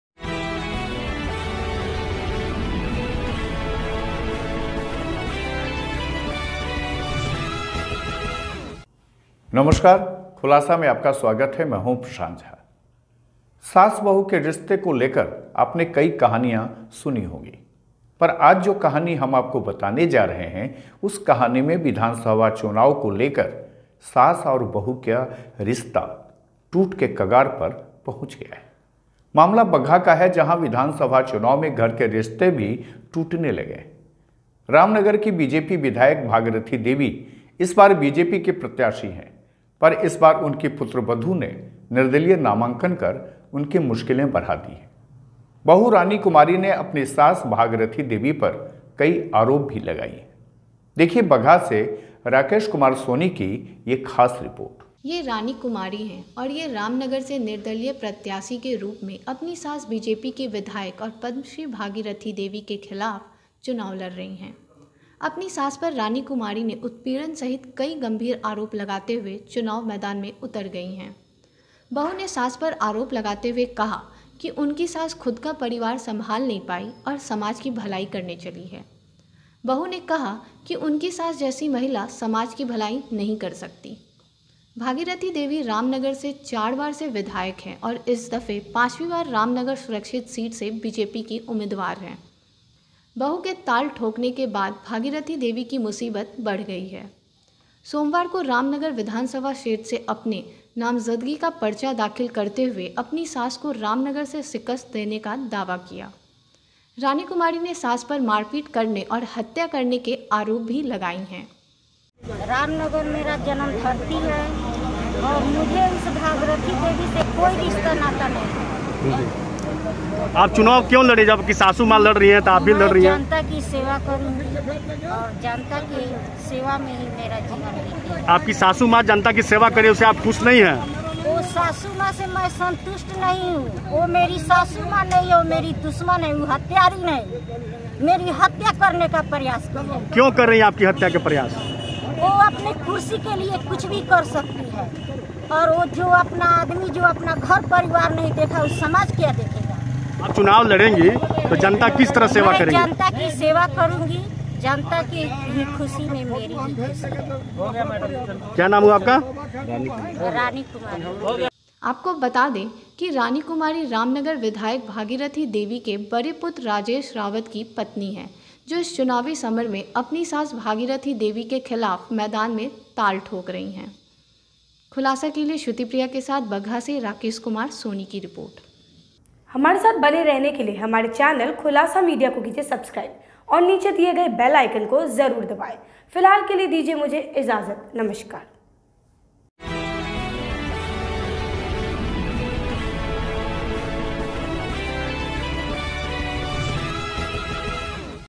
रामनगर में सास के खिलाफ मैदान में उतरी बहू
बगहा से राकेश कुमार सोनी की रिपोर्ट

ये रानी कुमारी है और ये रामनगर से निर्दलीय  प्रत्याशी के रूप में अपनी सास बीजेपी की विधायक और पद्मश्री भागीरथी देवी के खिलाफ चुनाव लड़ रही है ... अपनी सास पर रानी कुमारी ने उत्पीडन सहित कई गंभीर आरोप लगाते हुए चुनाव मैदान में उतर गयी है ...बहू ने  सास पर आरोप लगाते हुए कहा की उनकी सास खुद का  परिवार  संभाल नहीं पायी और समाज की भलाई करने चली है ... बहू ने कहा की  उनकी सास जैसी महिला समाज की भलाई नहीं कर सकती  । भागीरथी देवी राम नगर से  चार बार से विधायक हैं और इस दफे पांचवी बार रामनगर सुरक्षित सीट से बीजेपी की उम्मीदवार हैं। बहू के ताल ठोकने के बाद भागीरथी देवी की मुसीबत बढ़ गयी है. सोमवार को रामनगर विधानसभा क्षेत्र से अपने  नामजदगी का पर्चा दाखिल करते हुए अपनी सास को राम नगर से शिकस्त देने का दावा किया । रानी कुमारी ने सास पर मारपीट करने और हत्या करने के आरोप भी लगाई हैं .
 आपको बता दें की  रानी कुमारी रामनगर विधायक भागीरथी देवी के बड़े पुत्र राजेश रावत की पत्नी है जो इस चुनावी समर में अपनी सास भागीरथी देवी के खिलाफ मैदान में ताल ठोक रही  हैं.

KHULASA MEDIA
HOUSE NO – AB 47
SK PURI
BOARING ROAD
PATNA – 800001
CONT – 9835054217